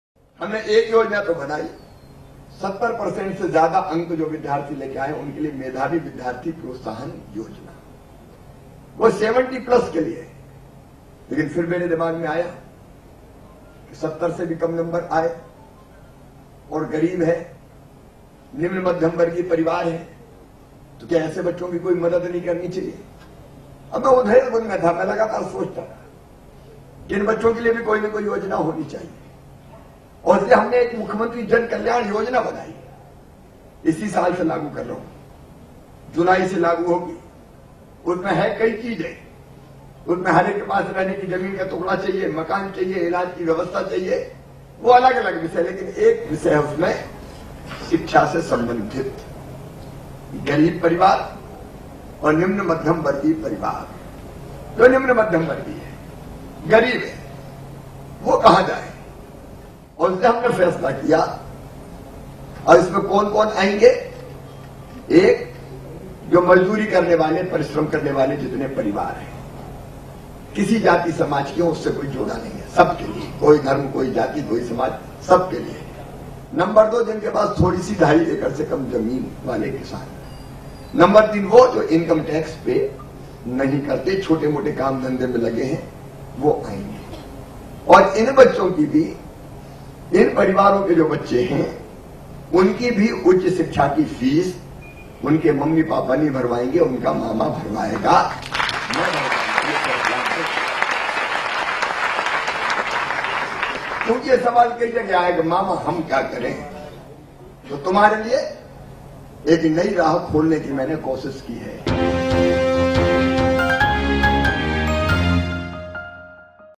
Mukhya Mantri Jan Kalyan Yojana will help students from BPL to get higher education: CM Chouhan
While addressing a gathering in Madhya Pradesh’s Bhopal, Chief Minister Shivraj Singh Chouhan said, “We are implementing a scheme, Mukhya Mantri Jan Kalyan Yojana, for kids, who come from the weaker section of the society.